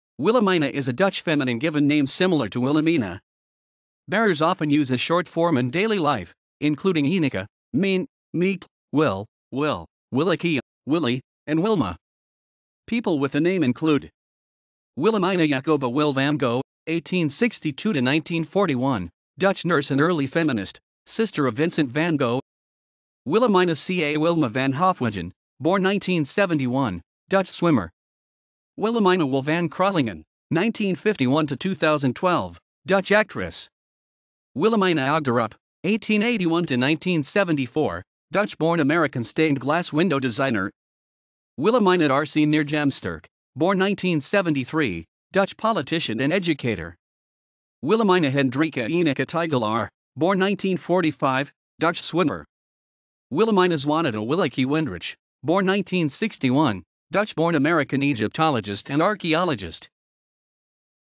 Willemina
Willemina is a Dutch feminine given name similar to Wilhelmina. Bearers often use a short form in daily life, including  Ineke, Mien, Miep, Wil, Will, Willeke, Willy, and Wilma. People with the name include:

Willemina Jacoba "Wil" van Gogh (1862–1941), Dutch nurse and early feminist, sister of Vincent van Gogh
Willemina C.A. "Wilma" van Hofwegen (born 1971), Dutch swimmer
Willemina "Will" van Kralingen (1951–2012), Dutch actress
Willemina Ogterop (1881–1974), Dutch-born American stained glass window designer
Willemina R.C. "Mirjam" Sterk (born 1973), Dutch politician and educator
Willemina Hendrika "Ineke" Tigelaar (born 1945), Dutch swimmer
Willemina Zwanida "Willeke" Wendrich (born 1961), Dutch-born American Egyptologist and archaeologist